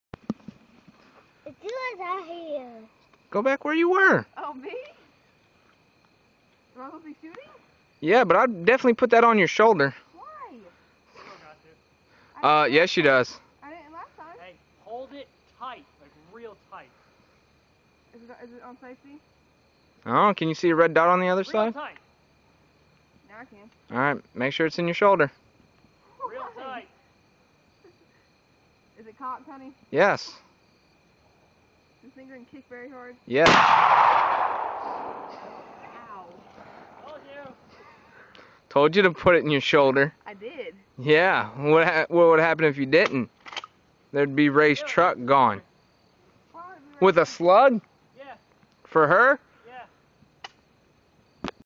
amanda 12ga slug
amanda shoots a 12ga